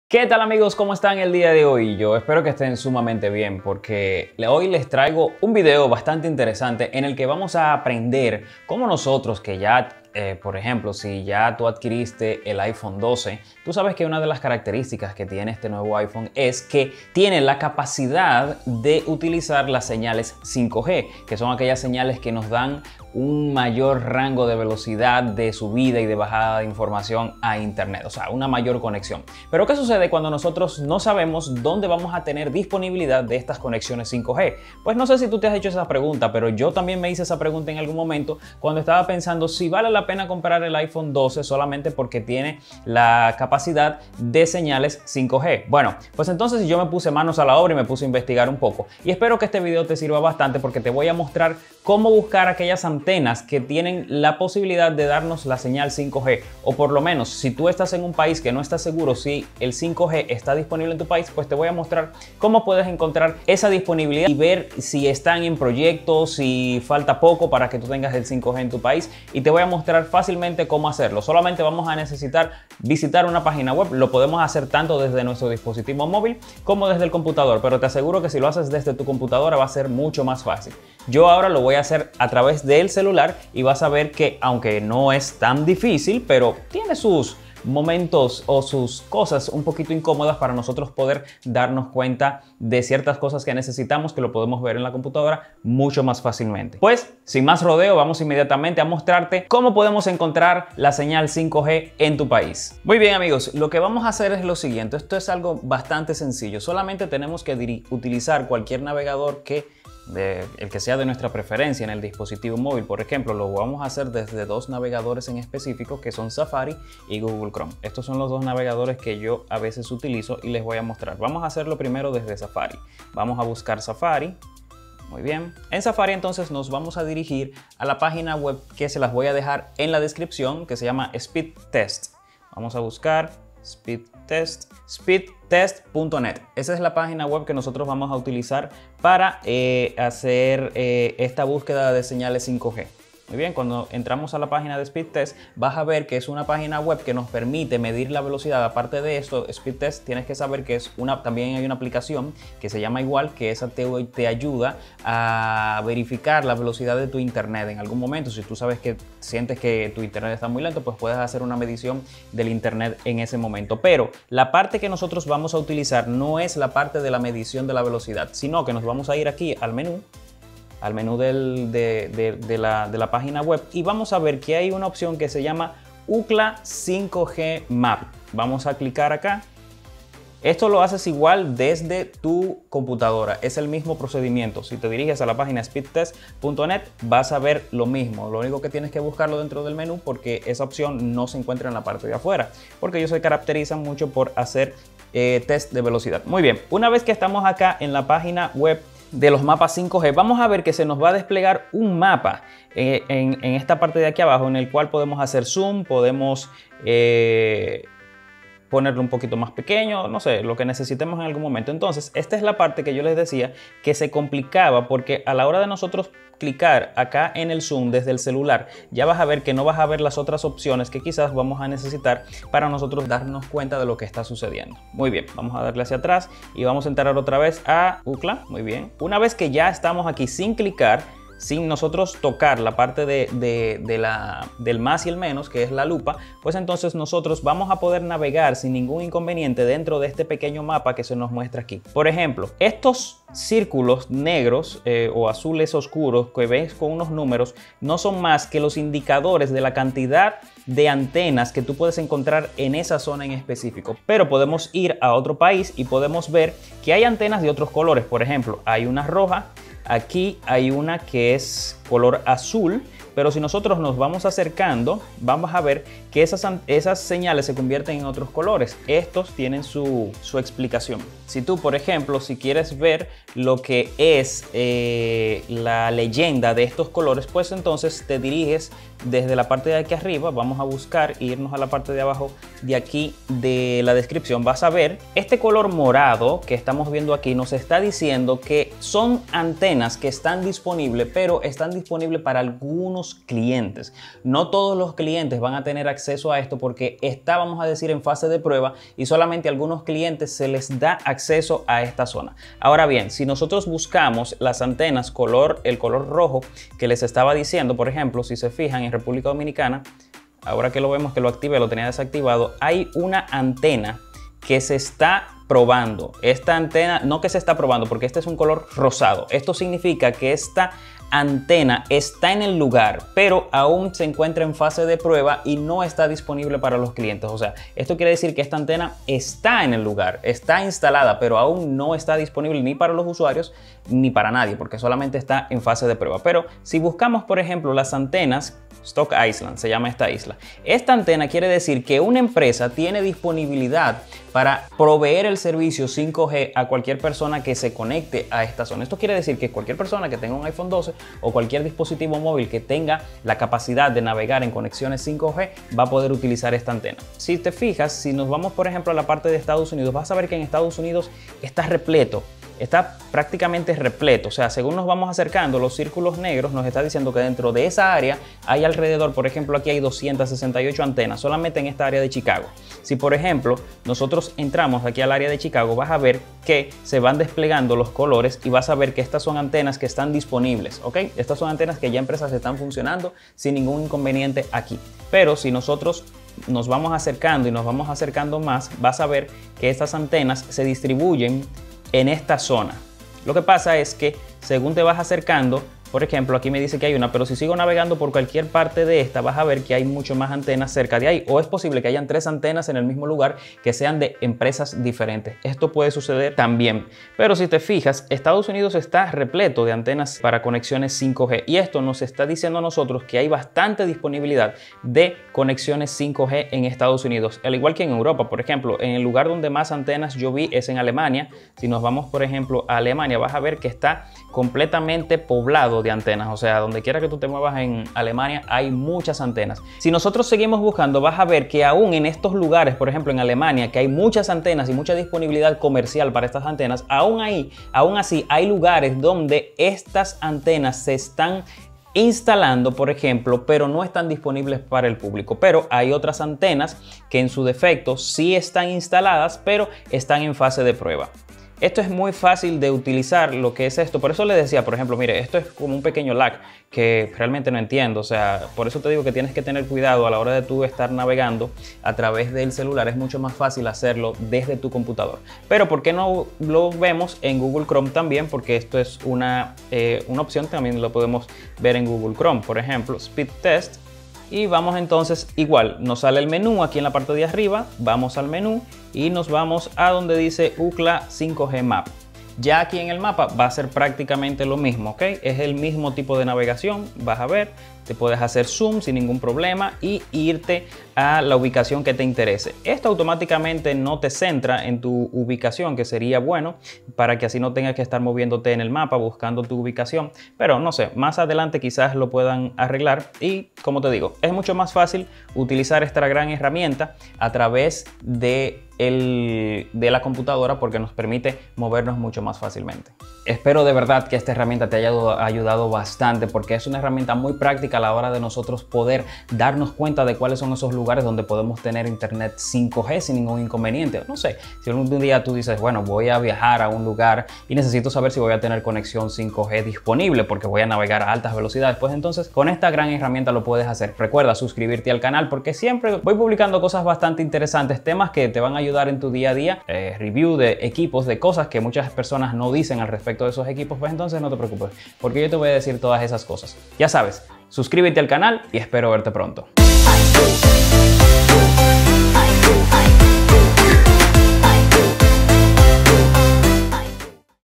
Encuentra SEÑALES 5G para tu IPHONE o ANDROID
En este video te muestro como puedes encontrar las señales 5G disponibles en tu país o ciuidad.

Encuentra móviles 5G en este enlace